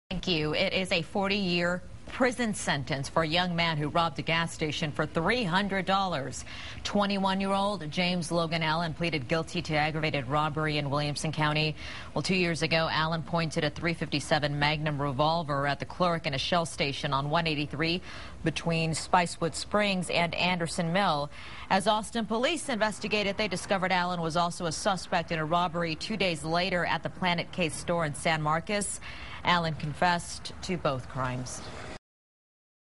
Robbery as a teen gets man sentenced
Man convicted by a Williamson County Judge is sentenced to 40 years in prison.